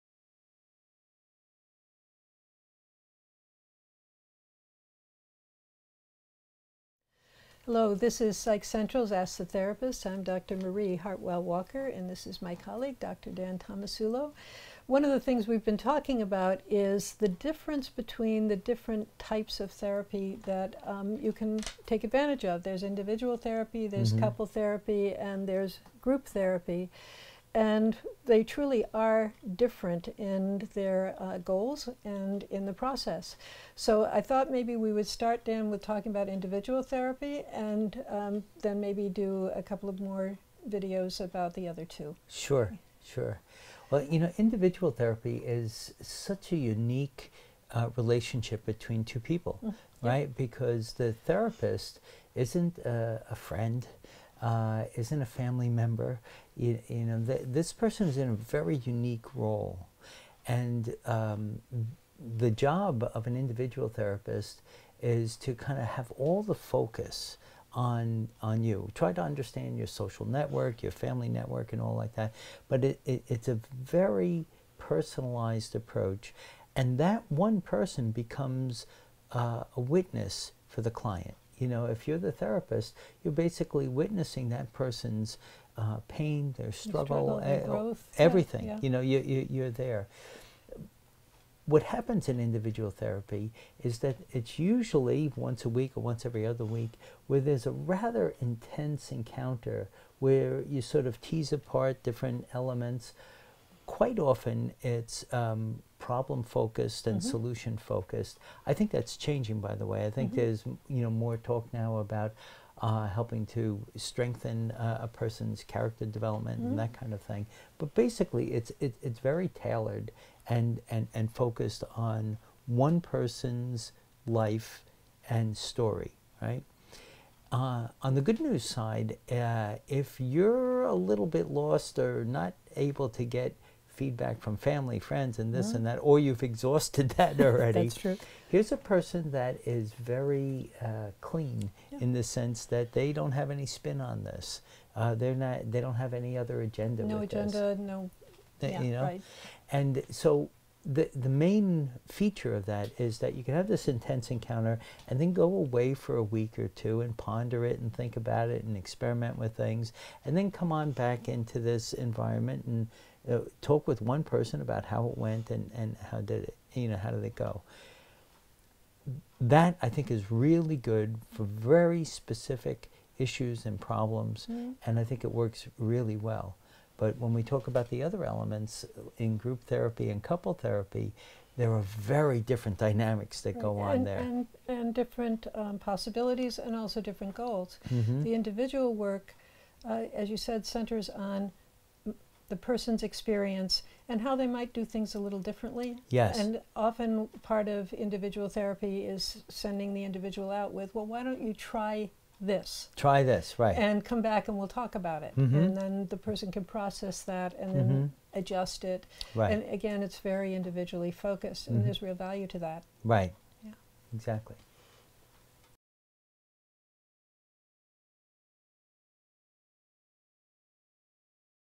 What is Psychotherapy? The Benefits of Individual Therapy
Ask the Therapists Marie Hartwell-Walker, Ed.D. and Daniel J. Tomasulo, Ph.D., TEP, MFA talk about the benefits of individual psychotherapy (also called counseling).